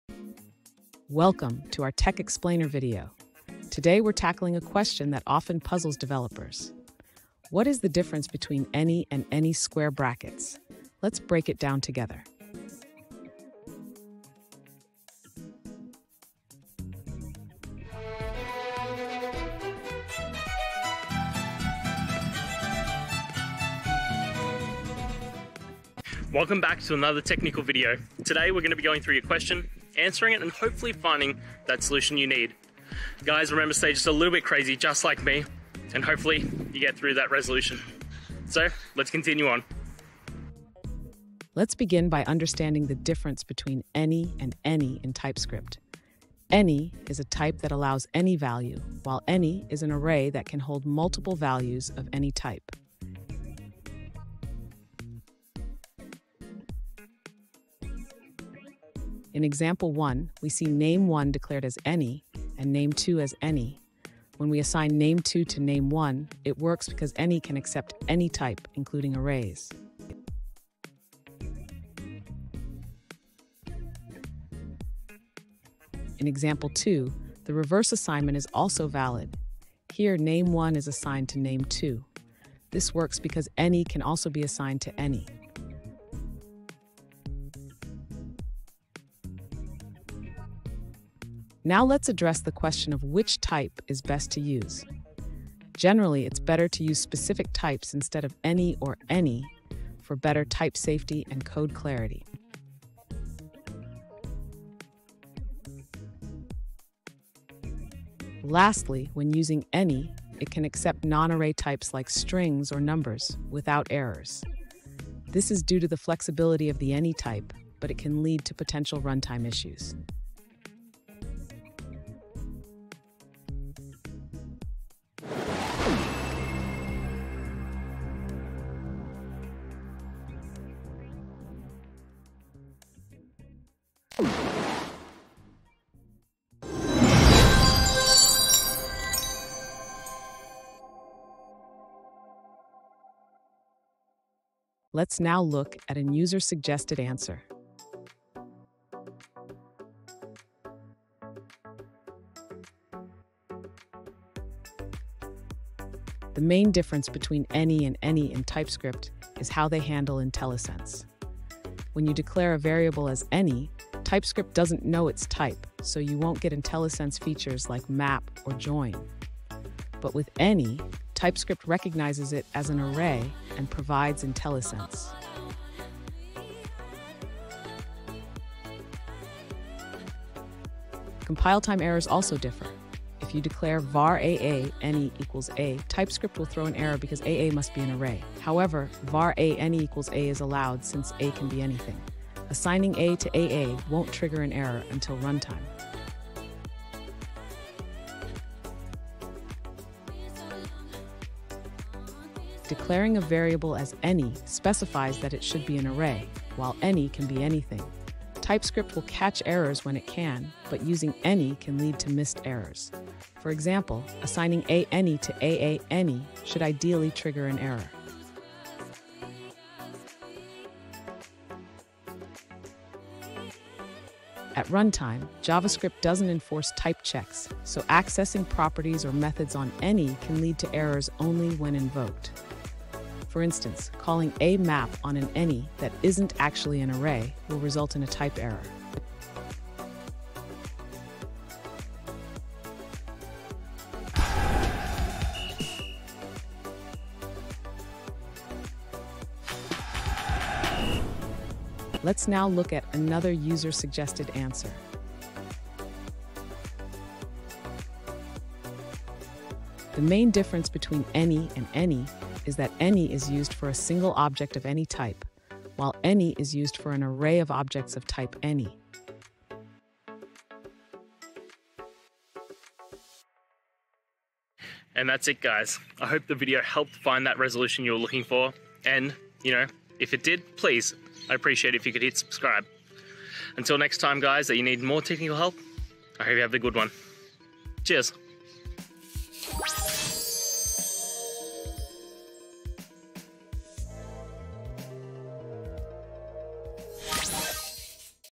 Understanding the Difference Between 'any' and 'any[]' in TypeScript
In this video, we delve into the nuances of TypeScript by exploring the differences between 'any' and 'any[]'. Understanding these two types is crucial for effective type management in your applications. We'll break down their definitions, use cases, and the implications of choosing one over the other, helping you write more robust and maintainable code. Join us as we clarify these concepts and enhance your TypeScript skills!

Today's Topic: Understanding the Difference Between 'any' and 'any[]' in TypeScript

Related to: typescript, anytype, any, differencebetweenanyandany,